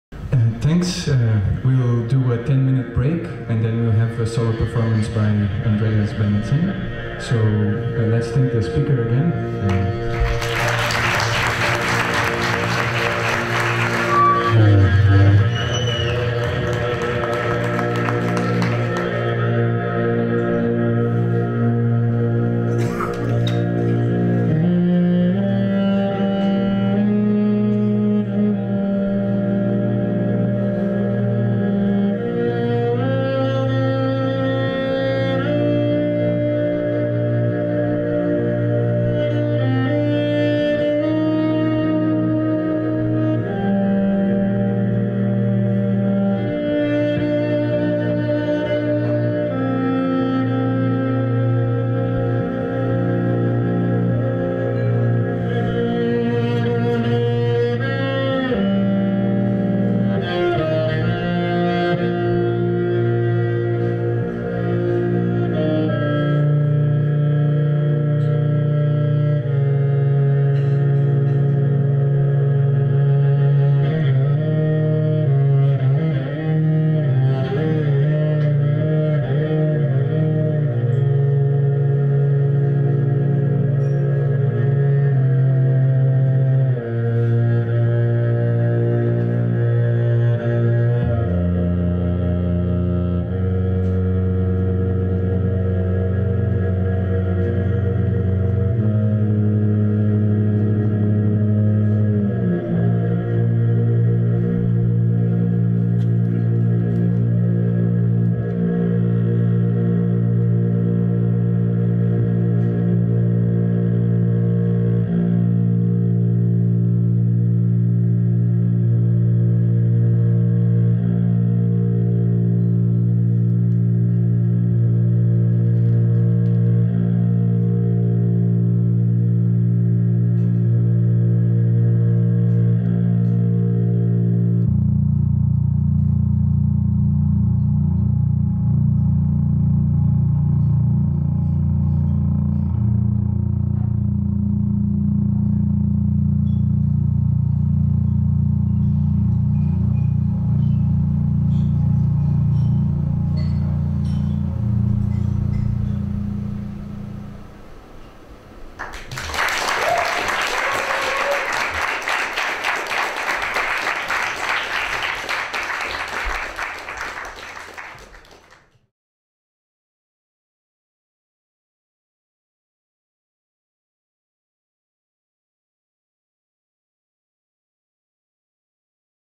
2011 maj 10_Andy Benz_Science & Cocktails 5
Andreas Barsøe Bennetzen,
Bassist, komponist, performer og iscenesætter
spillede ved arrangementet 'Science & Cocktails'
i 'Byens Lys', Christiania, d. 10. maj 2011.

Vist på Christiania TV d. 24. maj 2011.